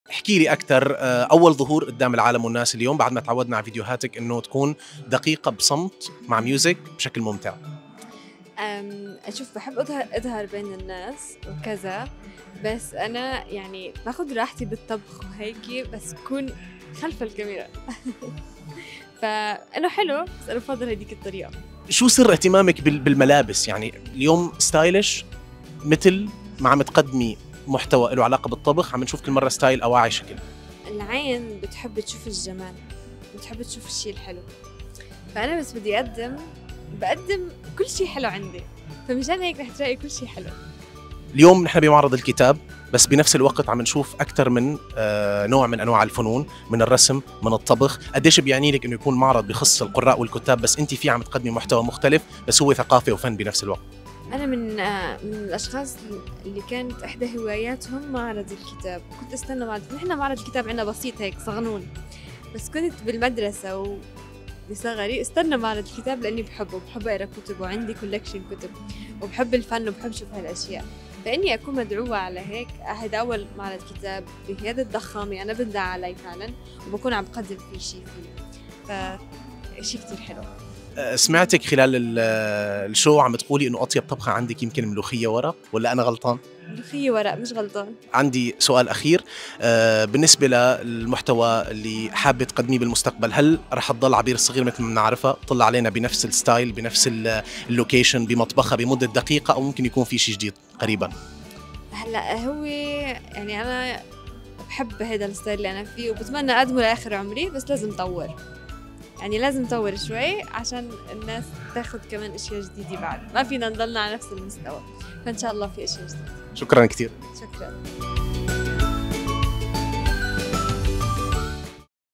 كيف علقت الشيف عبير الصغير على خلطها فن الطبخ بستايل الملابس ؟
عبير الصغير لإرم نيوز : معرض الكتاب هوايتي ، وكيف علقت على خلطها فن الطبخ بستايل الملابس ؟